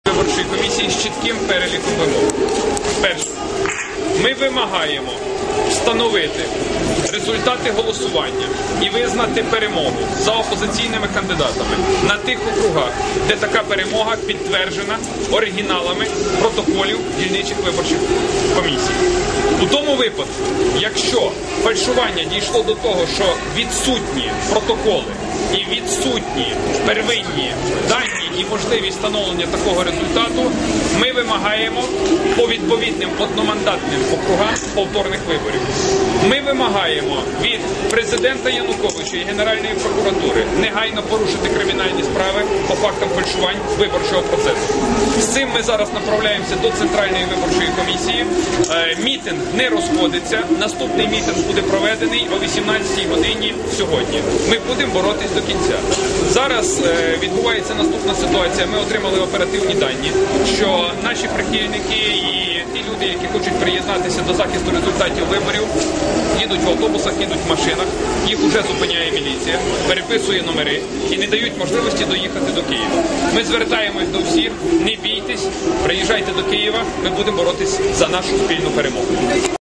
Оппозиция выставила ультиматум власти
Акция протеста возле Центризбиркома сегодня собрала около 7 тыс. людей. Лидеры оппозиции, которые возглавили митинг против фальсификаций, провели совещание прямо в подъезде жилого дома, находящегося возле ЦИК. После этого они направились в ЦИК с требованиями к власти.